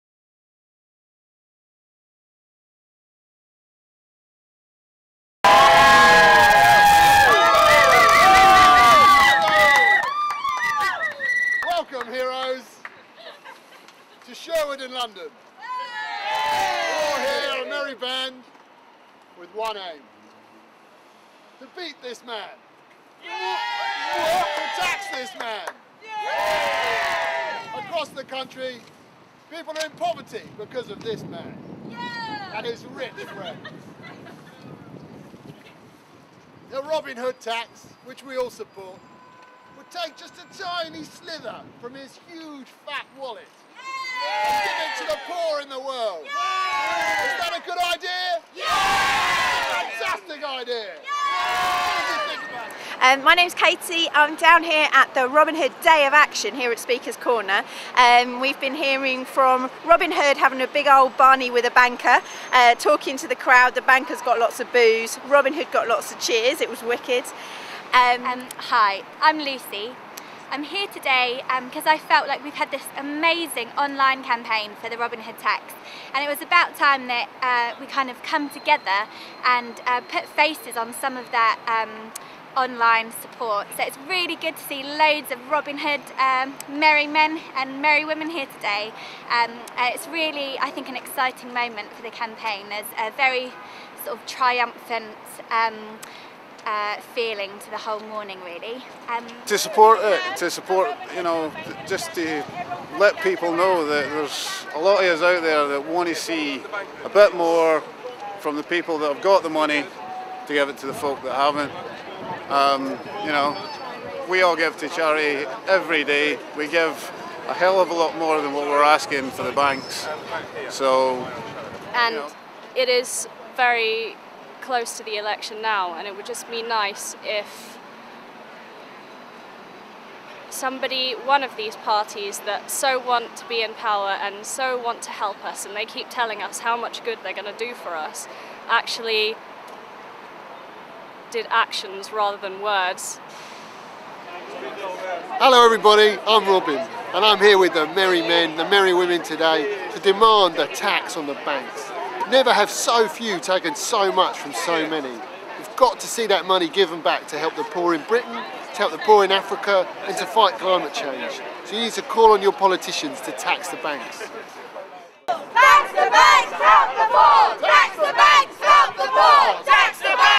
Robin Hood at Speakers' Corner
On Saturday 10th April the Robin Hood campaign held a day of action across the UK. The main event was at Speakers' Corner, Hyde Park, London. See what people thought of the campaign.